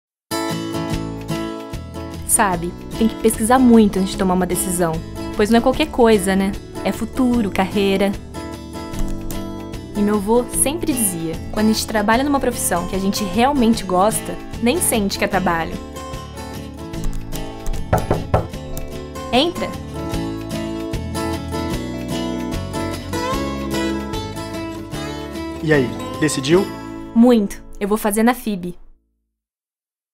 FIB 15 anos - Campanha Institucional 2013
Faculdades Integradas de Bauru. 15 anos de história. 15 anos de grandes conquistas. Uma grande faculdade para quem sonha grande.

Produção: Aero Comunicação e Studio F1ponto4.